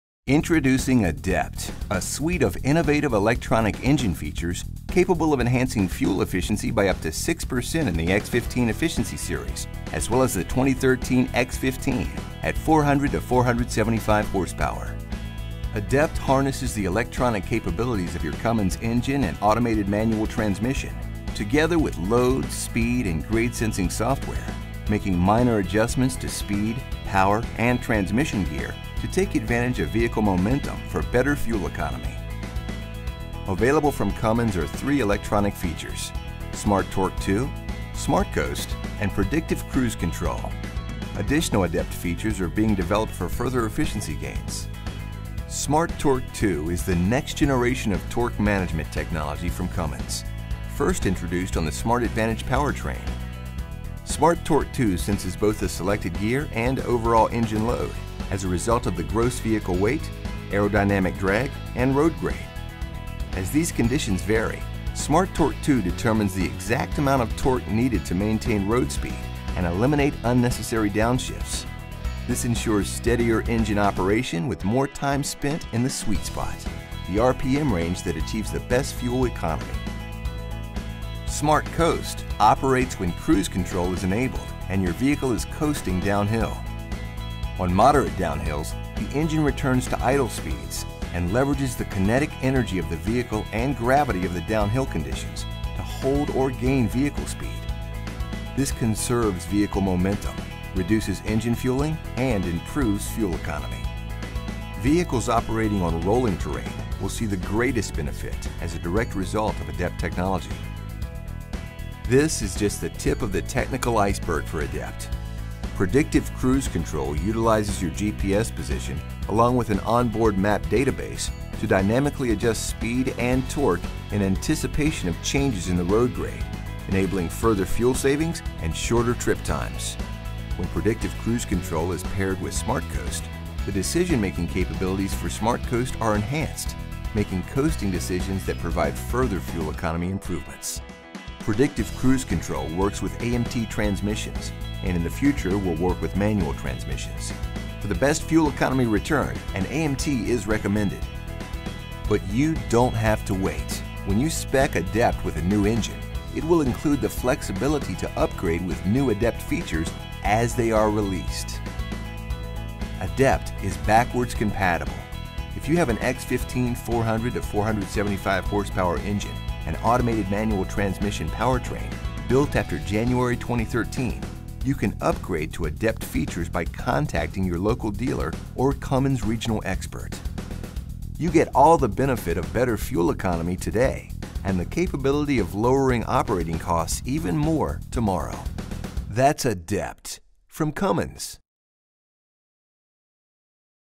Cummins X15 ADEPT
The new Cummins X15 platform carries forward the best-in-class attributes of the legendary ISX15,  now enhanced with increased thermal efficiency and the highest compression ratio in the industry, to improve engine response and reduce fuel consumption.

The two segment-optimized engines realize the full potential of the 15-litre platform. For line-haul and regional-haul applications, the X15 Efficiency Series with a 400 hp-to-500 hp  (298-373 kW), range offers an unmatched level of fuel economy with no compromise to engine response, with up to 2508 Nm of peak torque available at 1,100 rpm.

Excellent driveability is further enhanced with ADEPT,™ SmartCoast™ and Predictive Cruise Control (PCC) functions, to gain a further 6 percent fuel economy improvement while making every driver an expert.